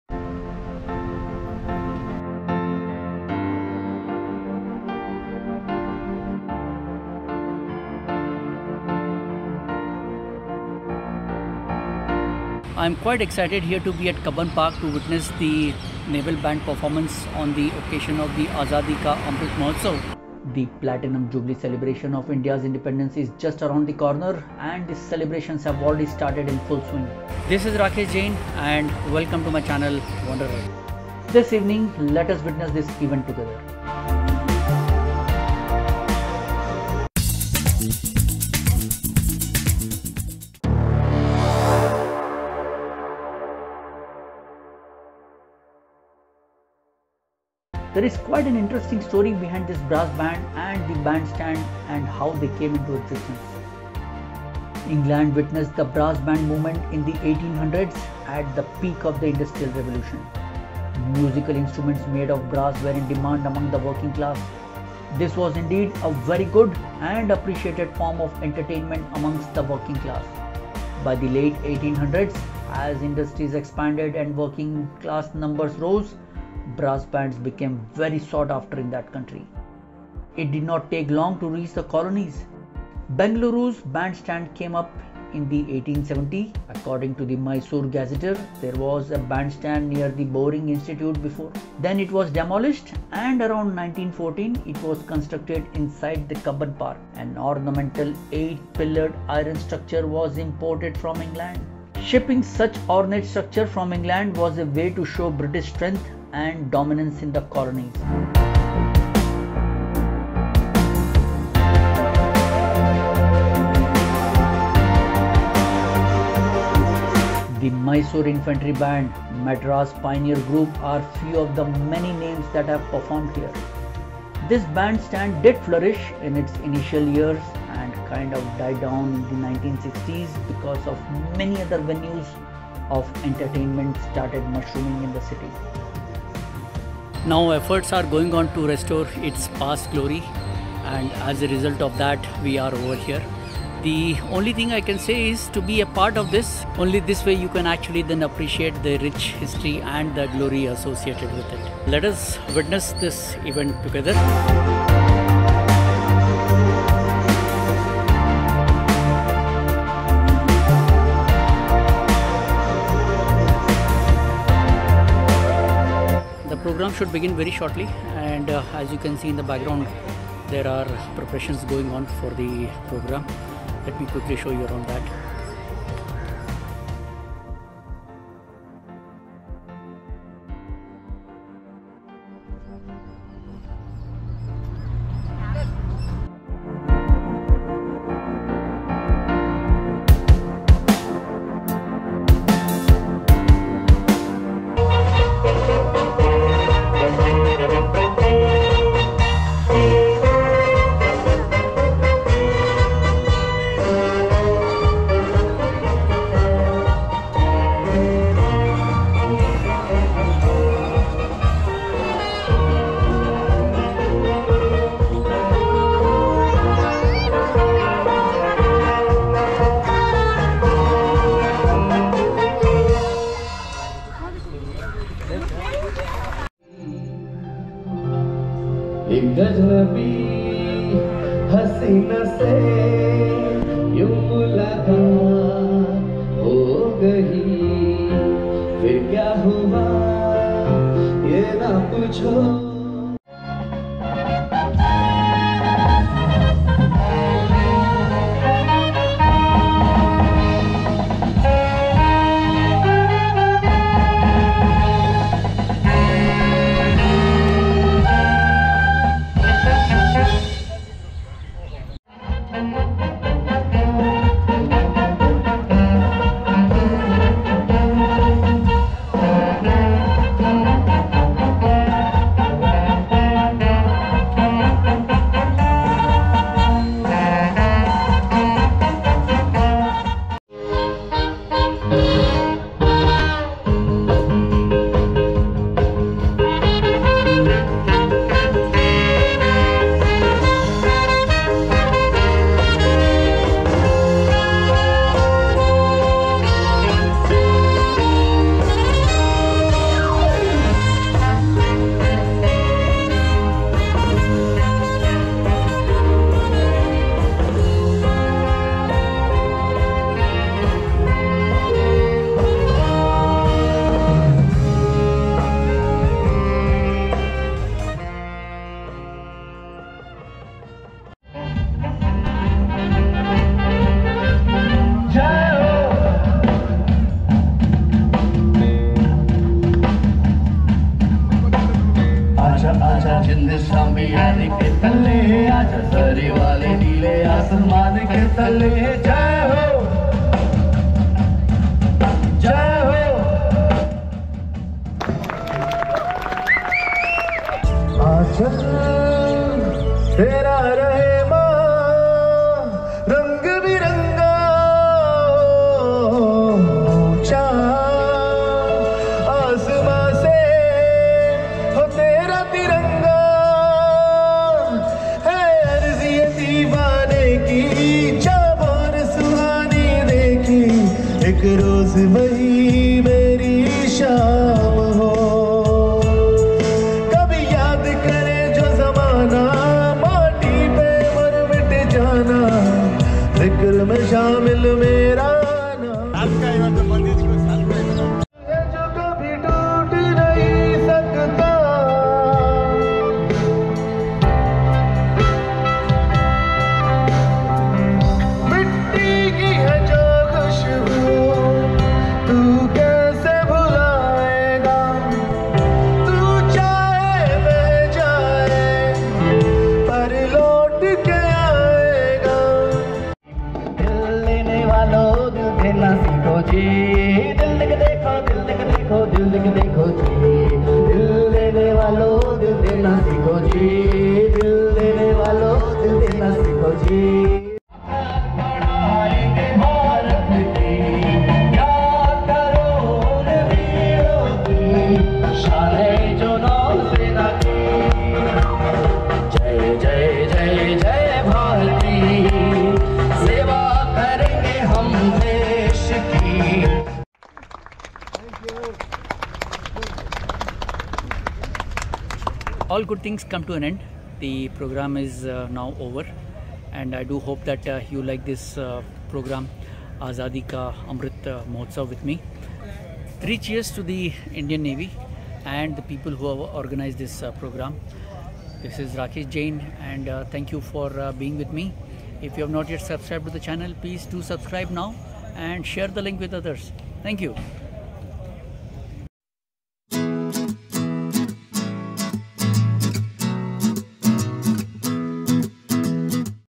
Band Stand @Cubbon Park, Bengaluru - A Rich History | Azadi Ka Amrit Mahotsava
Did you know that the Band Stand @Cubbon Park, Bengaluru had a rich history? When the music fills the air, the whole environment comes alive. Watch the glimpses of the performance by the Indian Navel Band on the occasion of Azadi Ka Amrit Mahotsava.

amritmahotsav, azaadikaamritmahotsav

00:00 Introduction
00:40 History behind Band Stand
03:10 Glimpses